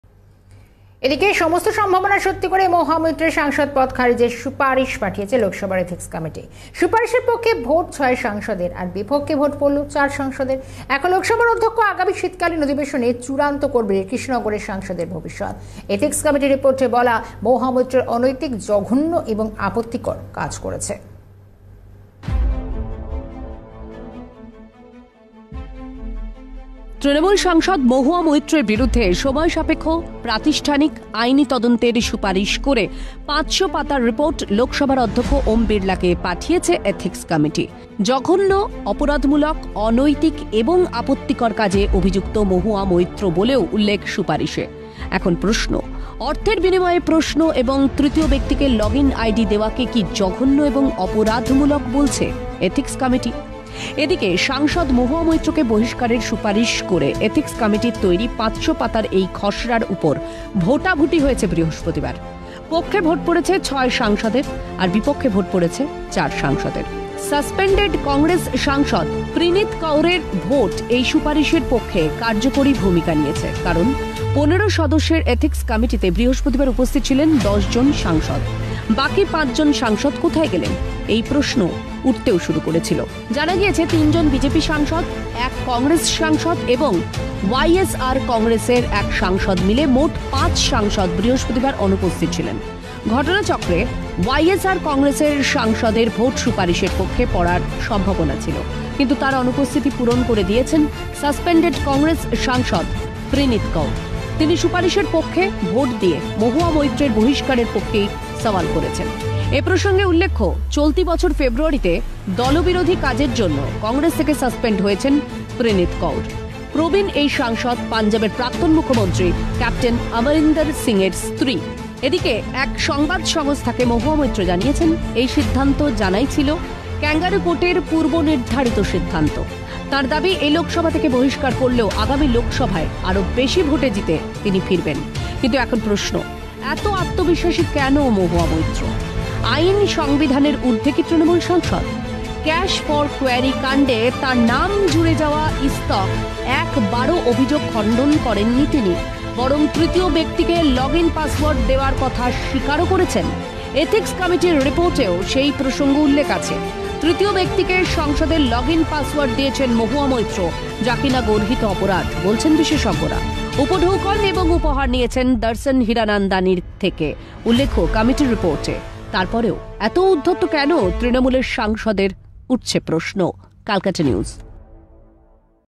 'জঘন্য, অপরাধমূলক, অনৈতিক', মহুয়ার বিরুদ্ধে সোচ্চার কমিটি | ক্যালকাটা নিউজ ডিজিটাল
'জঘন্য, অপরাধমূলক, অনৈতিক', মহুয়ার বিরুদ্ধে সোচ্চার কমিটি